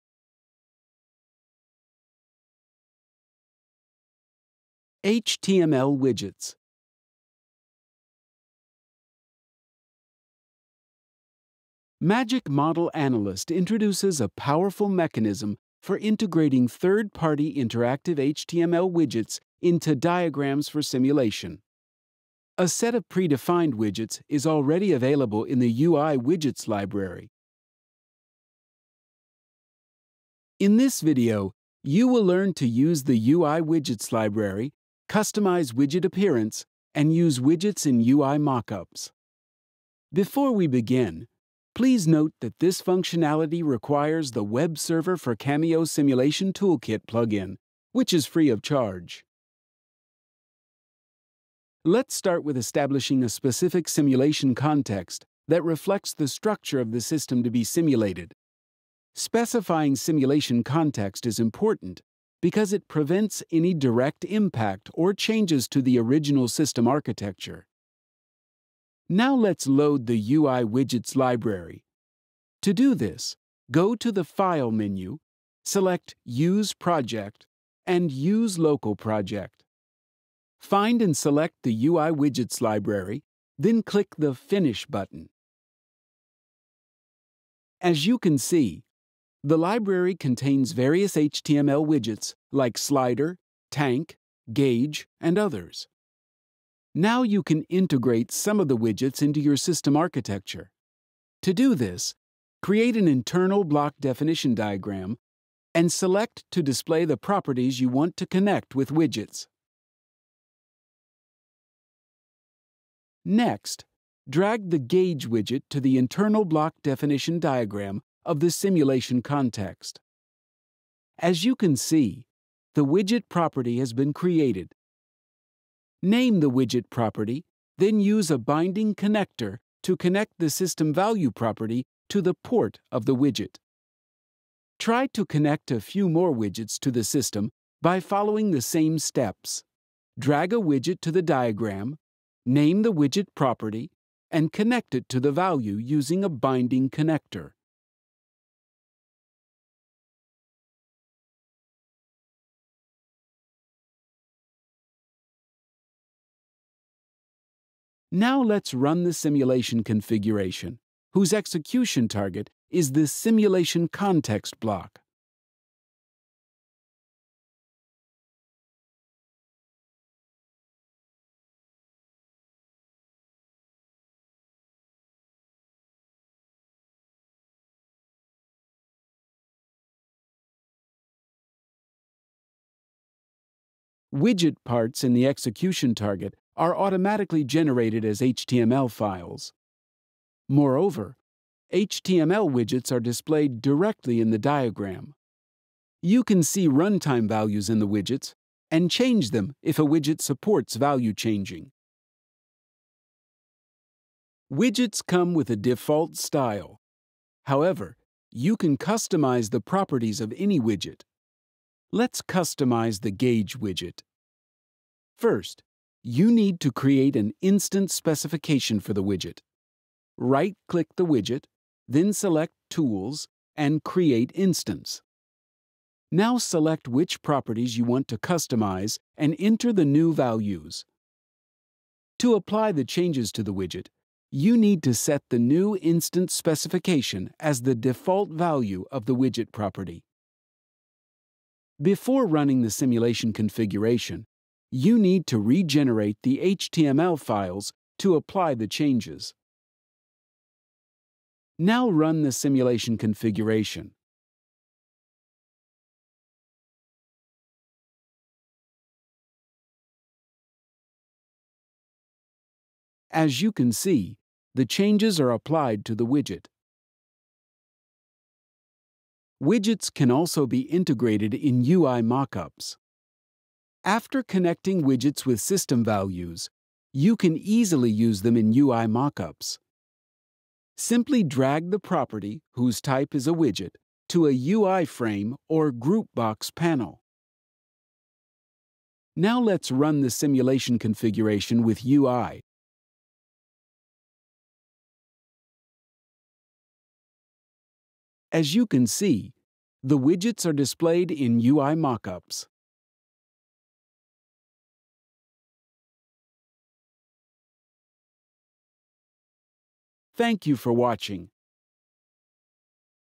How to Use Modern UI Widgets in SysML Simulation
This demonstration by R&D shows how to use modern HTML widgets for simulation UI in IBD and in  GUI modeling diagram. You can use and configure modern predefined widgets from widgets library and create custom one. Widgets can show results and control simulation.